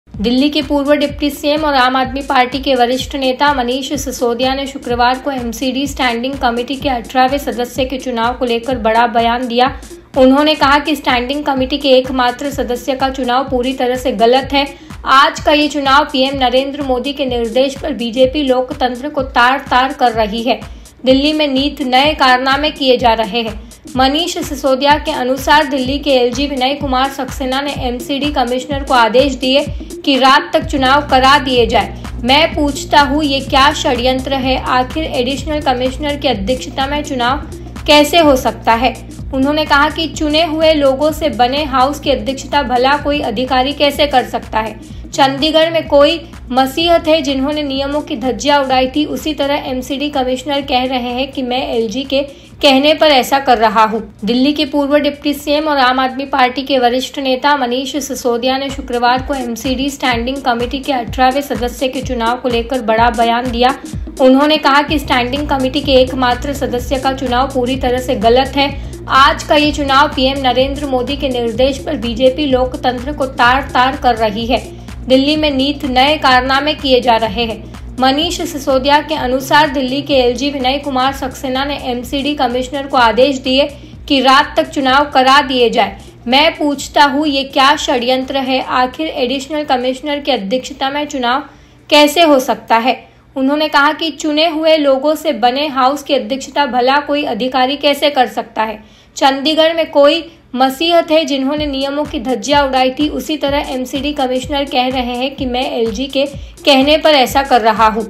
Manish Sisodia का ऐलान, MCD स्टैंडिंग कमेटी चुनाव में हिस्सा नहीं लेगी AAP
दिल्ली के पूर्व  डिप्टी सीएम और आम आदमी पार्टी के वरिष्ठ नेता मनीष सिसोदिया ने शुक्रवार को एमसीडी स्टैंडिंग कमेटी के 18वें सदस्य के चुनाव को लेकर बड़ा बयान दिया है. उन्होंने कहा कि स्टैंडिंग कमेटी के एक मात्र सदस्य का चुनाव पूरी तरह से गलत है. आज का ये चुनाव पीएम नरेंद्र मोदी के निर्देश पर बीजेपी लोकतंत्र को तार तार कर रही है. दिल्ली में नित नए कारनामे किए जा रहे हैंमनीष सिसोदिया के अनुसार दिल्ली के एलजी विनय कुमार सक्सेना ने एमसीडी कमिश्नर को आदेश दिए कि रात तक चुनाव करा दिएं जाएं. मैं, पूछता हूं- ये क्या षड्यंत्र है? आखिर  एडिशनल कमिश्नर की अध्यक्षता में चुनाव कैसे हो सकता है?